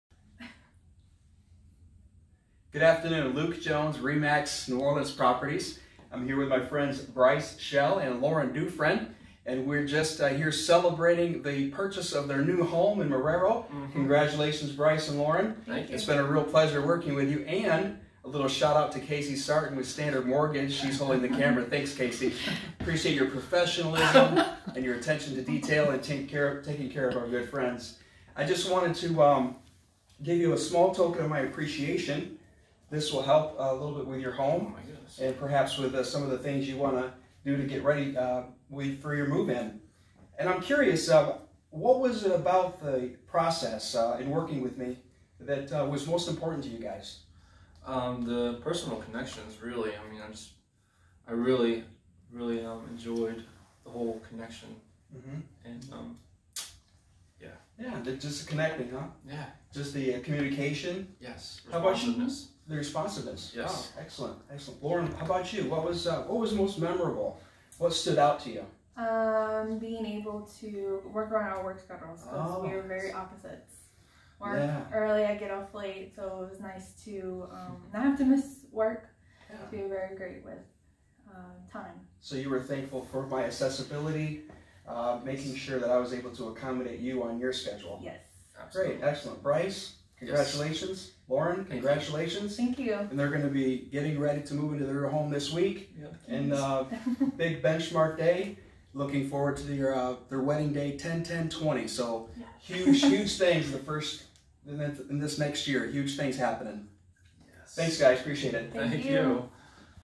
What Are First Time Home Buyers Saying?
When Bryce and Lauren, first-time homeowners, hired me to help find their first home they appreciated how I connected with them early on in the process. I understood where they wanted to live and what was important to them in their first home. They liked that my response time was fast and that I was very accommodating with appointments around their demanding work schedules. Lauren said, "We literally could not have done this without you! We appreciate all your help!"